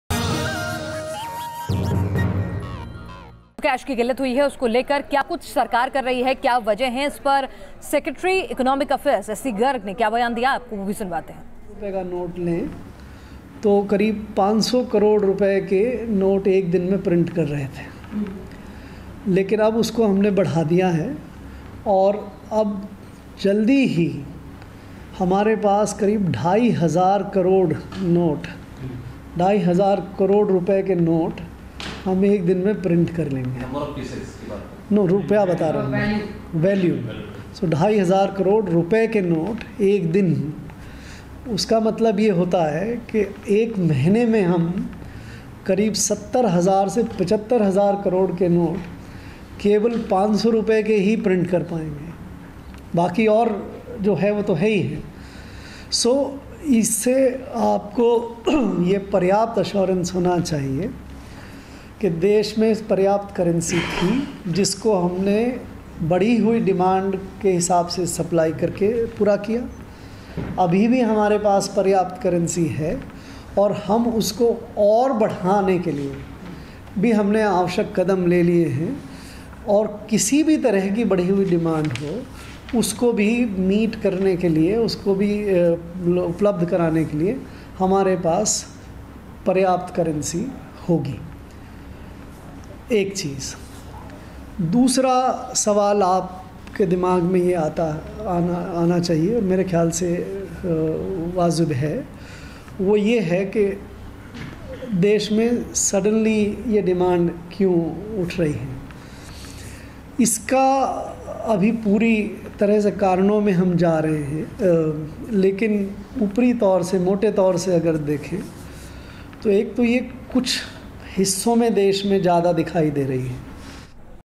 Cash Crunch: Secretary of Dept of Economic Affairs says we will now print more notes
SC Garg, the Secretary of Department of Economic Affairs had said that because of the cash crunch, now the steps to print Rs 500 notes will raise. The production will now raise about 5 times the normal production. He said in coming days, the Rs 500 notes will regulate at about 2500 crore per day and 70,000-75,000 crore in a month. Garg said, “We print about 500 cr of Rs 500 notes per day. We have taken steps to raise this production 5 times. In next couple of days, we'll have supply of about 2500 cr of Rs 500 notes per day. In a month, supply would be about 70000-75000 cr.”